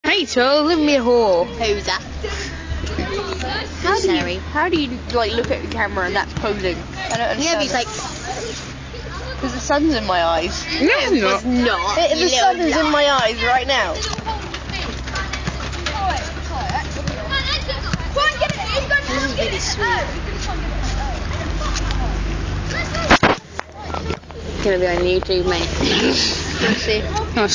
a lunch moment
me courtney and rach eating lunch at school + some random year 7 boys playing footy against the science block :]